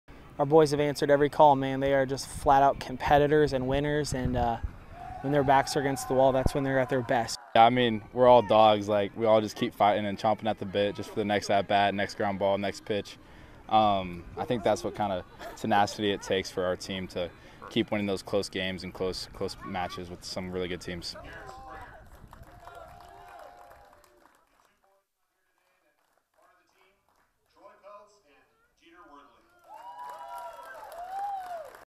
Interviews With Carpet Lands Mychal Lanik and Garrett Springer
1011 Sports talks to Carpet Land's (Lincoln East Baseball) Mychal Lanik and Garrett Springer after there 2023 State Legion Baseball Championship victory.